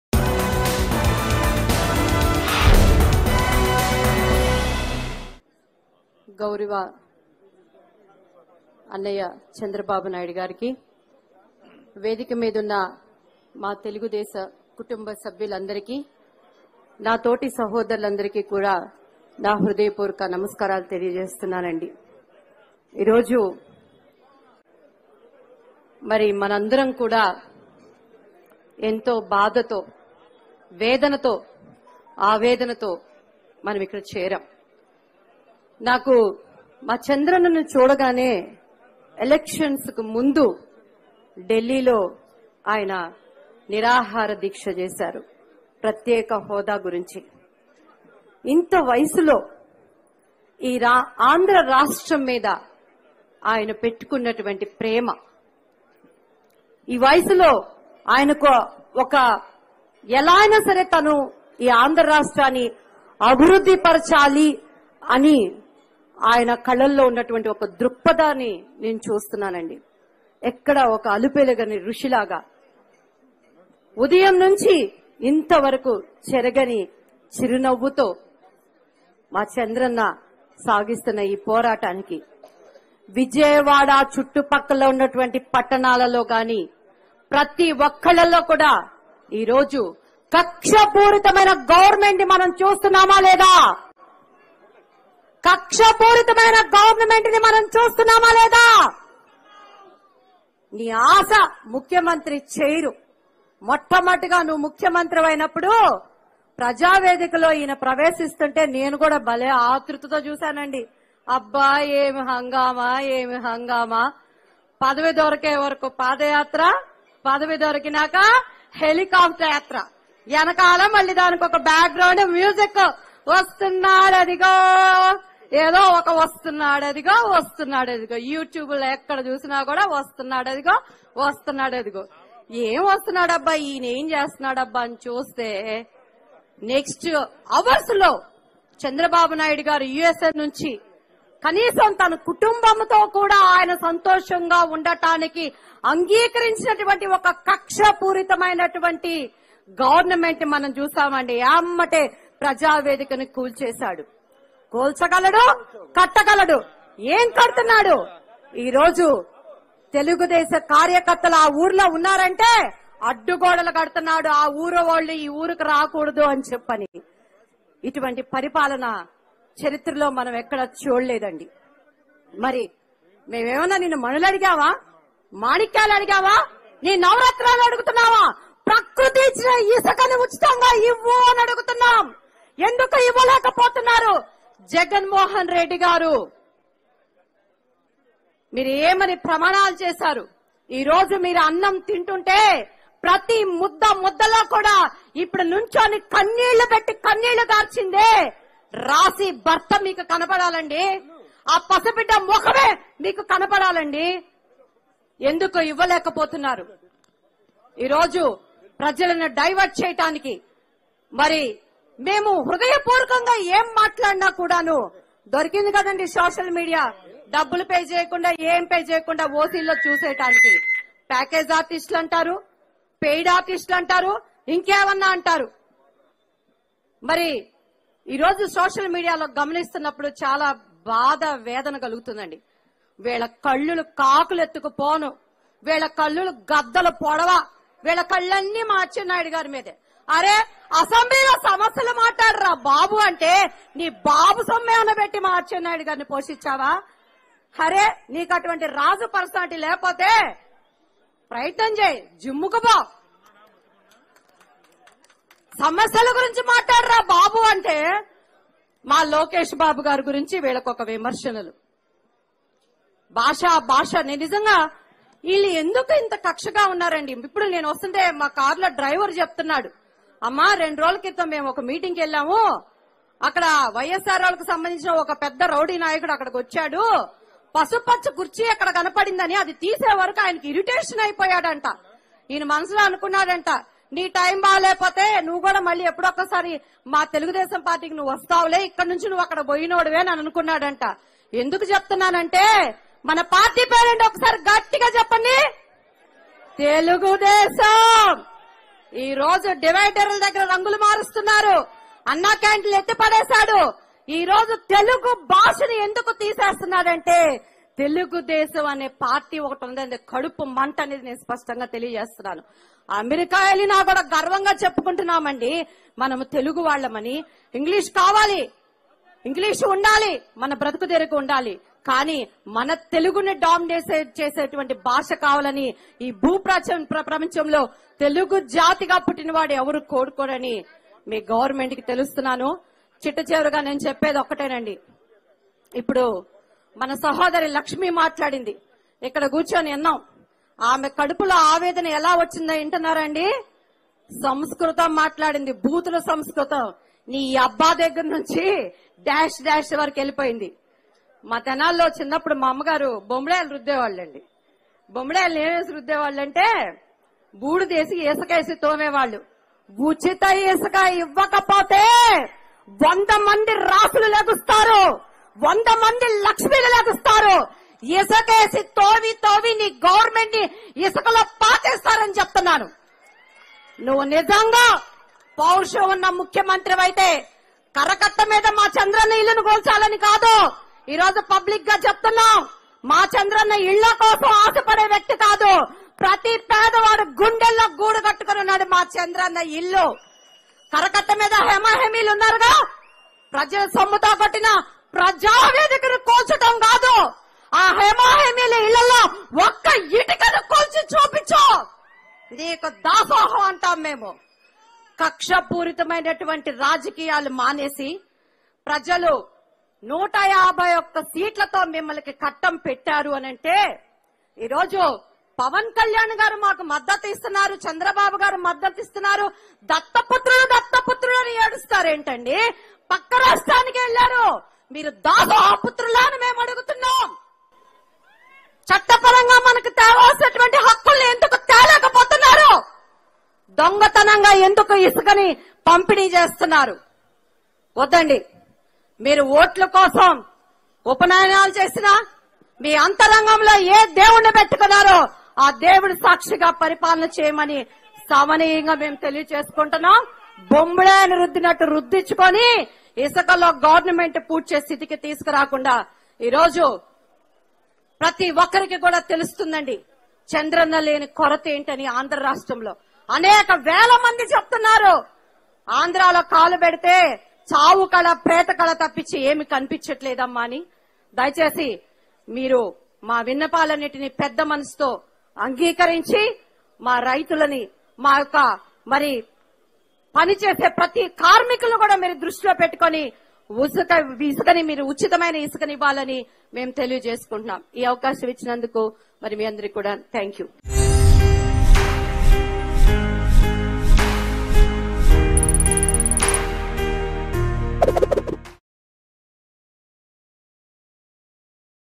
Divyavani Satires On CM YS Jagan Over Sand Mafia Issue | ChandrababuNaiduProtest | Mango News
Divyavani Satires On CM YS Jagan Over Sand Mafia Issue.